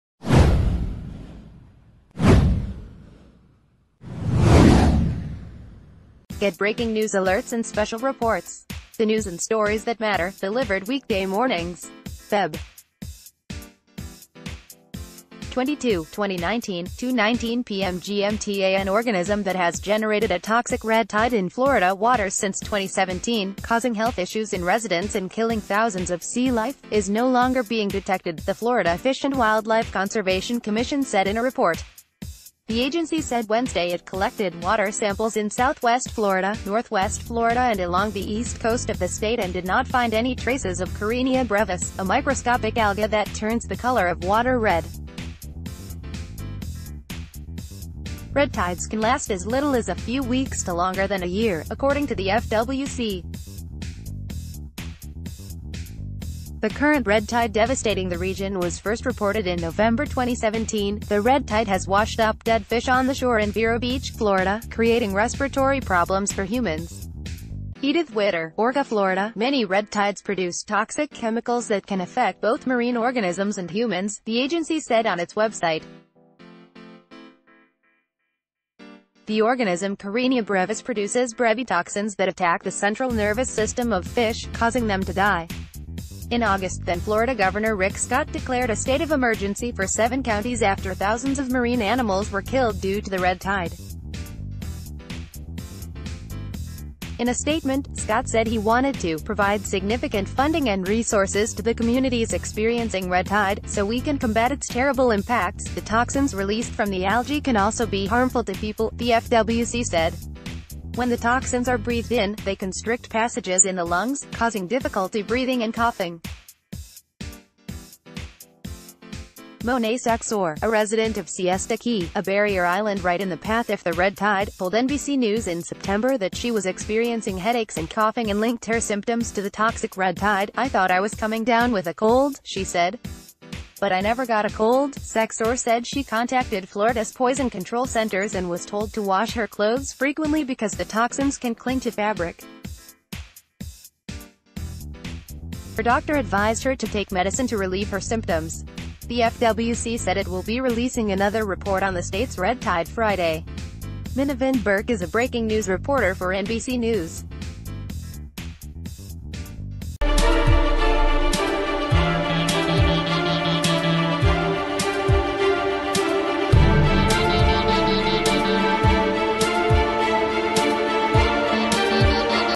Toxic red tide in Florida not detected in waters
Get breaking news alerts and special reports. The news and stories that matter, delivered weekday mornings. Feb. 22, 2019, 2:19 PM GMTAn organism that has generated a toxic red tide in Florida waters since 2017, causing health issues in residents and killing thousands of sea life, is no longer being detected, the Florida Fish and Wildlife Conservation Commission said in a report. The agency said Wednesday it collected water samples in southwest Florida, northwest Florida and along the East Coast...